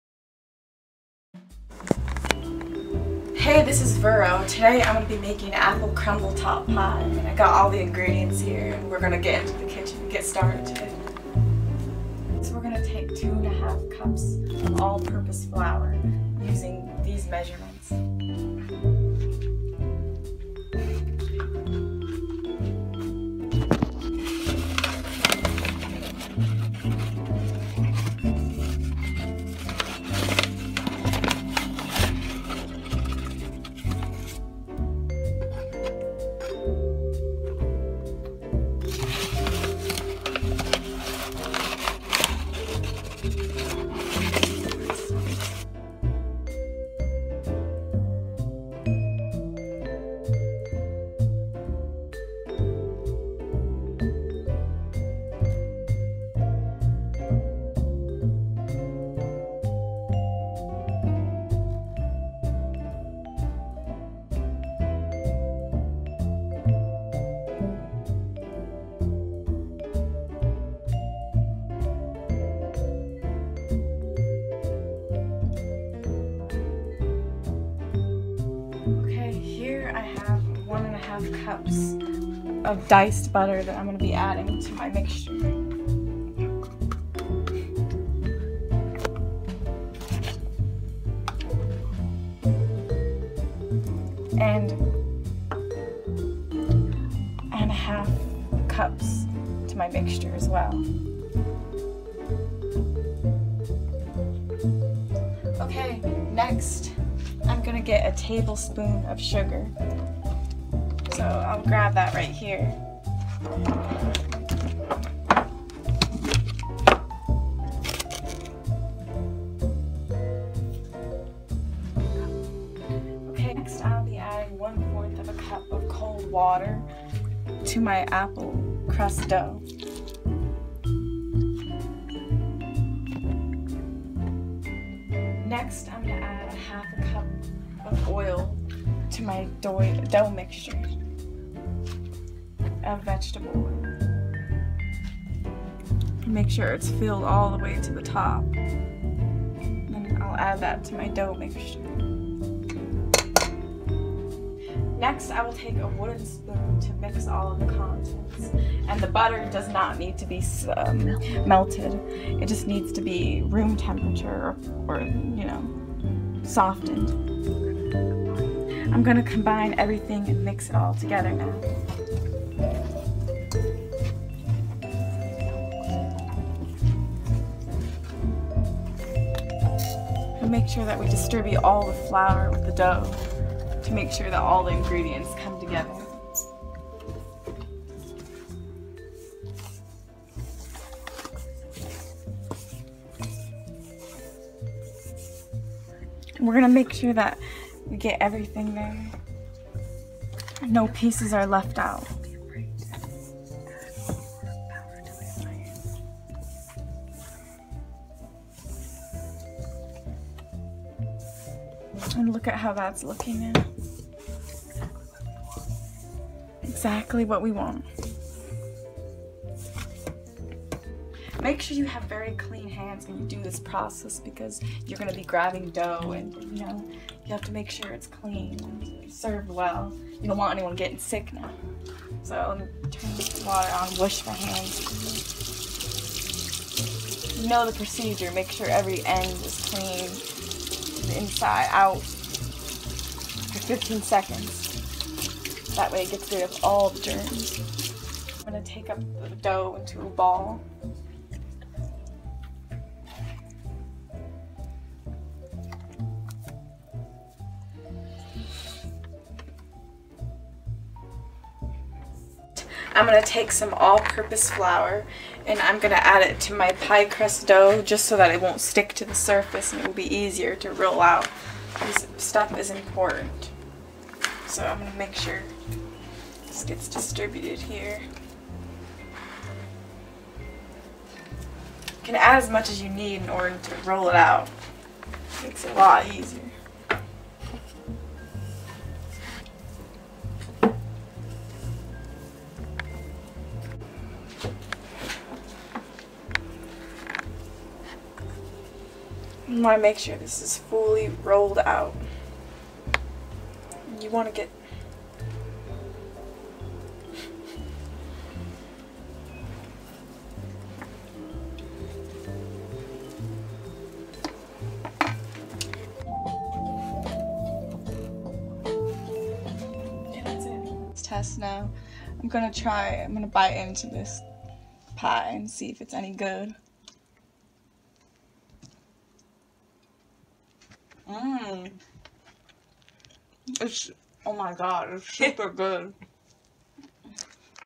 Cooking apple crumble top pie
Apple pie crumb-top recipe:
Ingredients (The pie crust):
*2 cups 1/2 all purpose flour
*teaspoon table salt
*Tablespoon sugar
*12 tablespoon (1 1/2 sticks ) cold
unsalted butter, cut into 1/4 - inch pieces
*1/2 cup vegetable oil, cut into 4 pieces
*1/4 cup cold water

The crumb top:
1/4 cup sugar
1/4 teaspoon ground cinnamon
1 1/2 cups cored, pared , sliced, tart apples
2 4 1/4 inch UNBAKED pastry shells
1 tablespoon sugar
2 tablespoons all-purpose flour
1 tablespoon butter or margarine
Directions: Pre-heat oven to 375 degrees.
*In a separate bowl combine all the ingredients for the crumb top recipe.
*In a separate bowl combine all your ingredients for the pie crust.
For the pie crust- roll dough into a ball and sprinkle a little flour on your cooking surface.
Roll your dough completely flat.
*Take out a standard sized pie pan and place dough at the bottom of your pan.
*Add a couple of apples on top of your pie dough.
*Last- take your crumb top pie mixture and sprinkle your crumb top on your pie.
*Place in oven to cook.